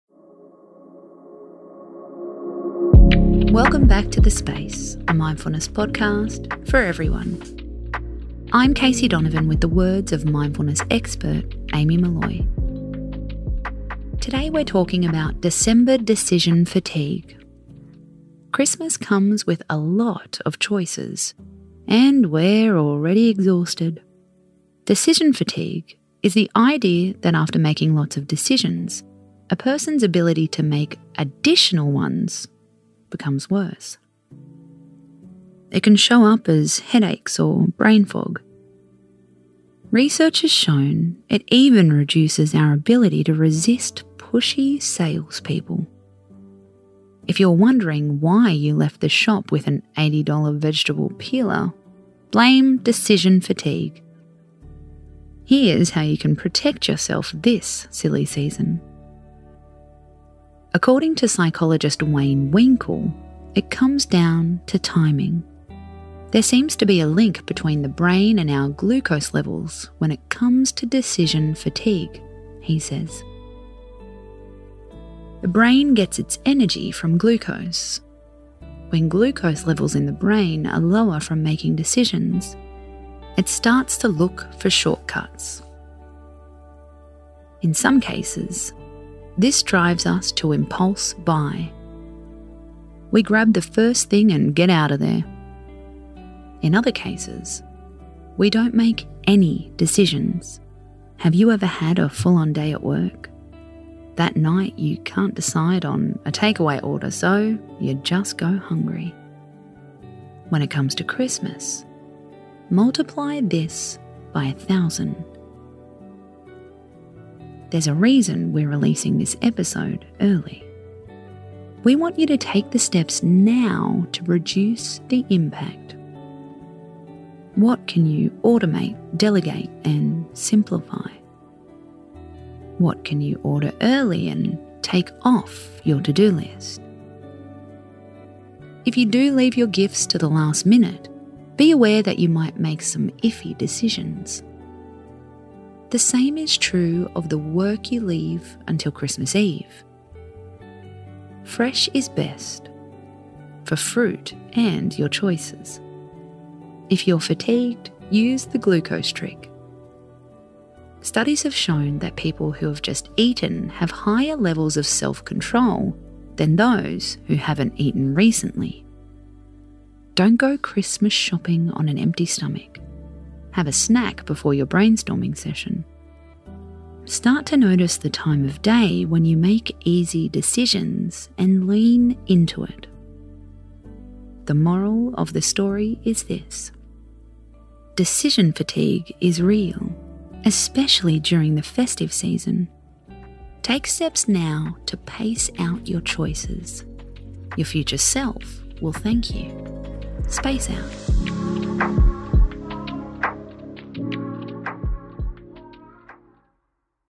Avoiding 'decision fatigue' this silly season
In this episode, we’re helping you to fight that December decision-fatigue. Know the feeling? You're so overwhelmed by all the decisions you need to make, that you don't feel like you can make a single one. We've got tips on how to get through it this silly season.

LINKS

CREDITS
Host: Casey Donovan @caseydonovan88
Producers: Elise Cooper and Anna Henvest